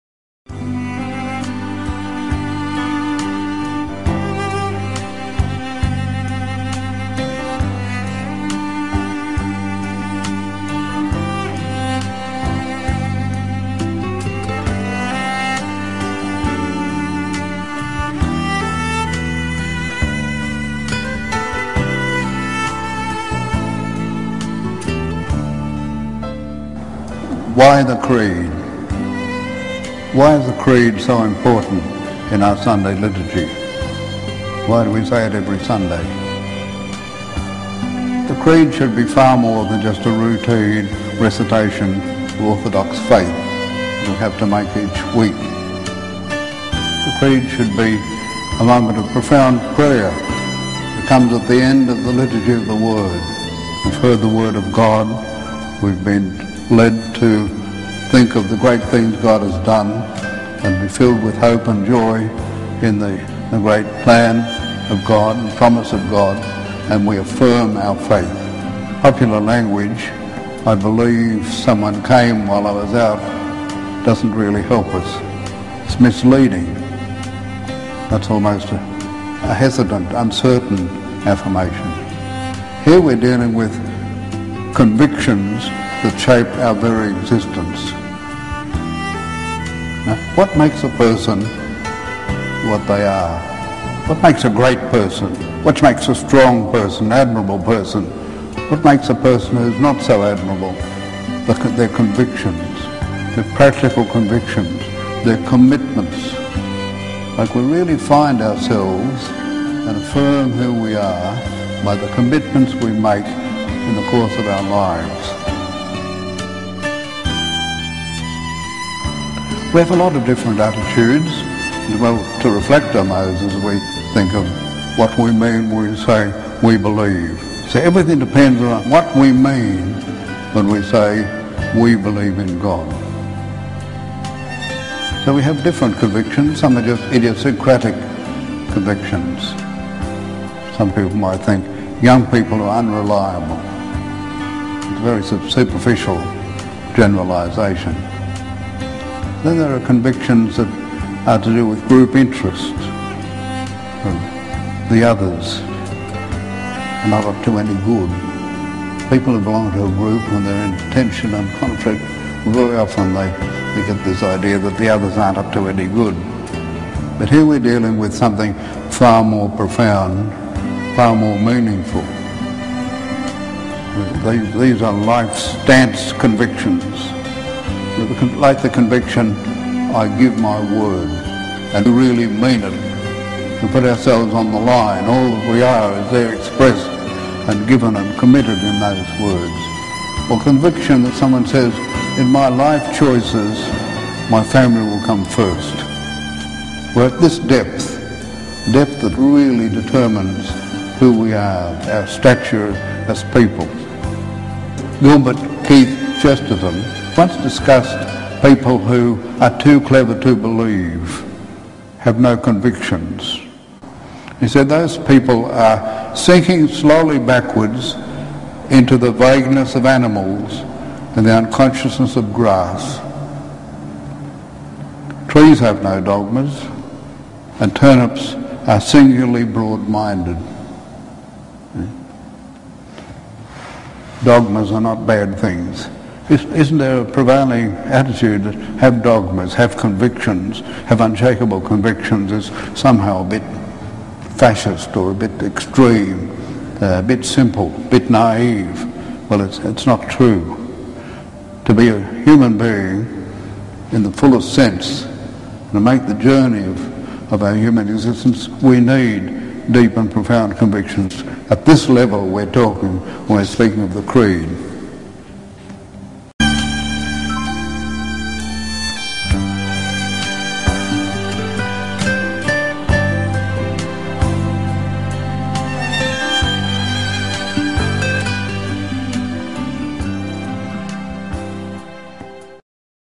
El Credo 1.1 Reflexiones para el año de la Fe (John Thornhill, sm)
Comenzamos, en este año de la fe, utilizando el material que generosamente John Thornhill nos ha permitido, para acompañar nuestra reflexión sobre el Credo como el Papa Benedicto nos ha invitado. Es una serie verdaderamente buena con una teología sólida y una espiritualidad profunda. En este capítulo introductorio: ¿Qué sentido tiene recitar el Credo? ¿Qué significa creer? ¿Cuál es el papel de las convicciones en el sentido de la vida?